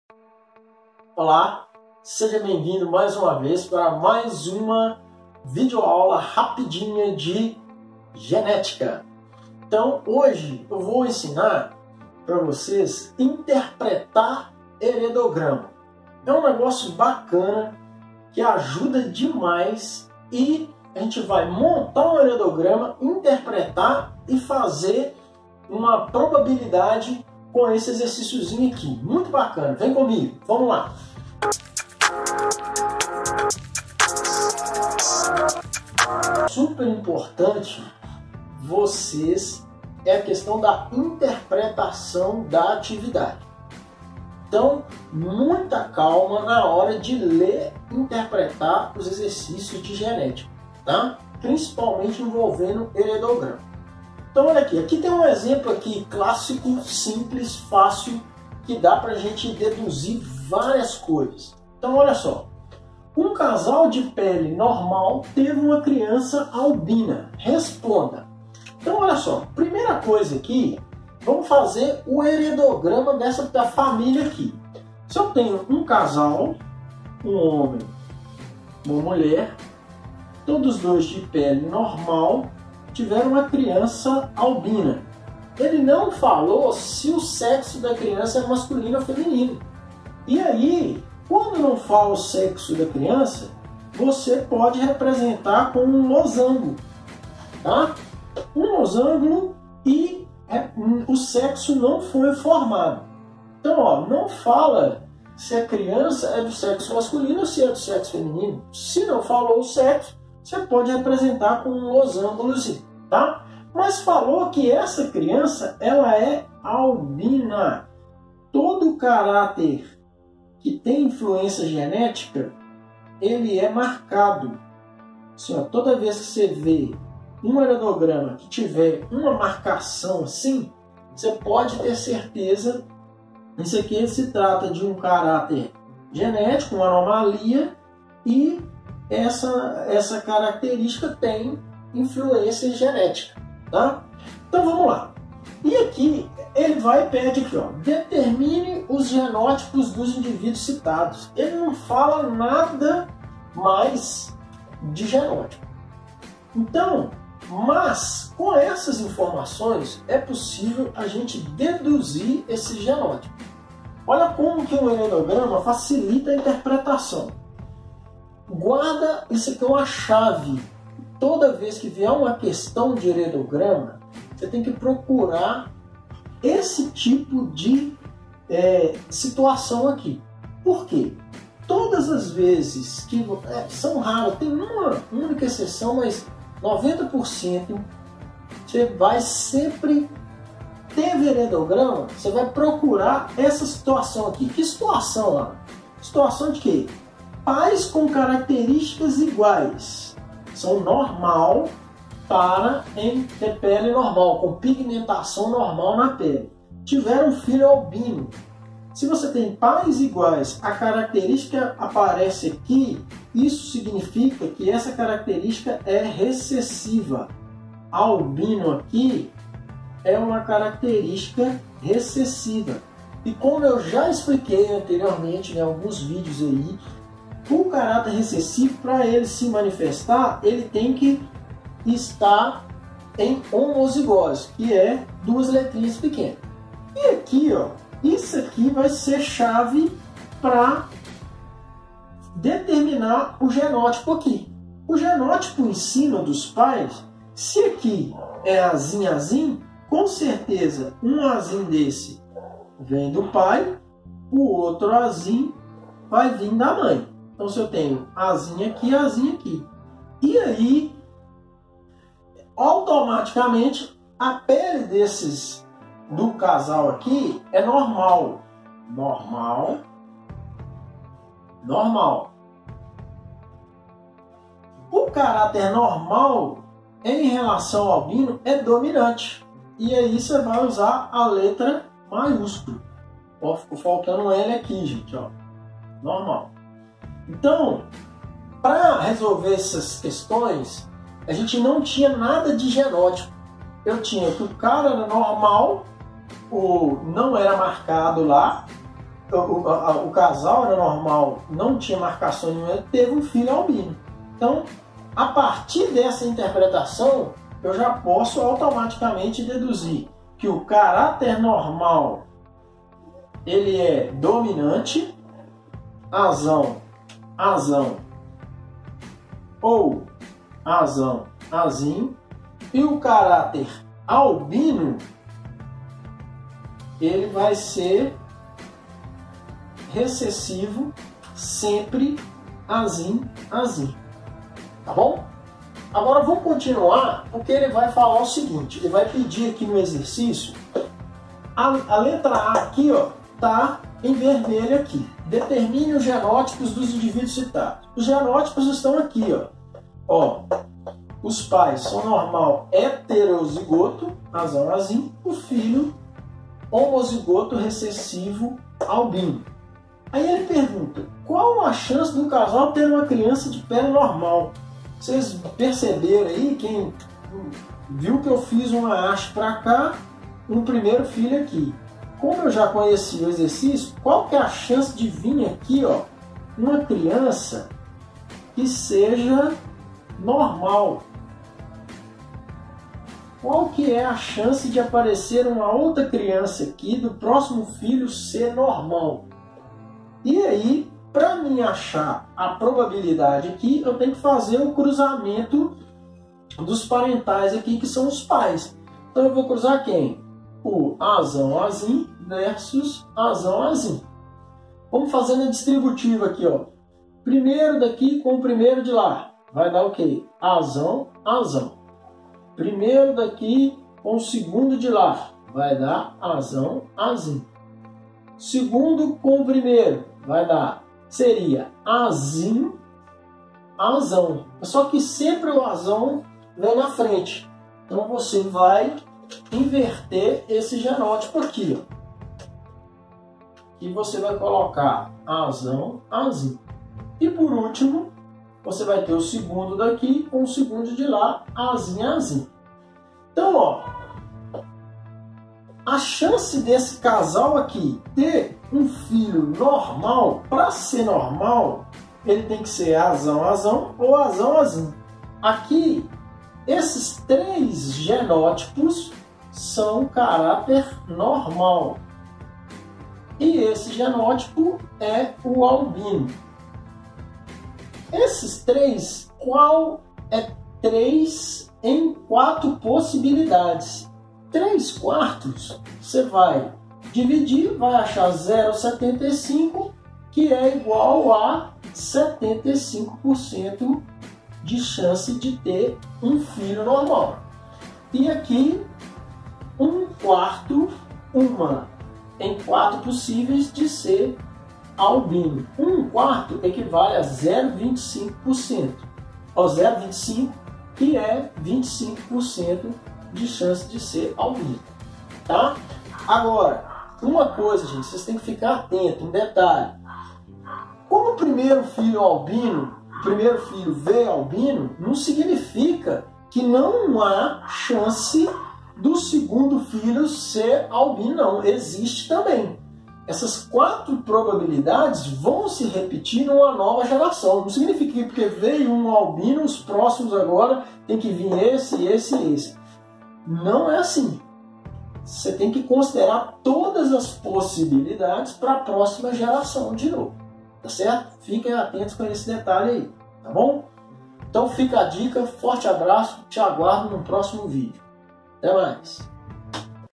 Interpretação de heredograma
Prof. ALAN TEMPONE
BIOLOGIA / Ensino Médio.

Construção e Interpretação de heredogramas.
Resolução de atividade de genética.
Dedução de genótipos a partir dos fenótipos.
Cálculo de probabilidades.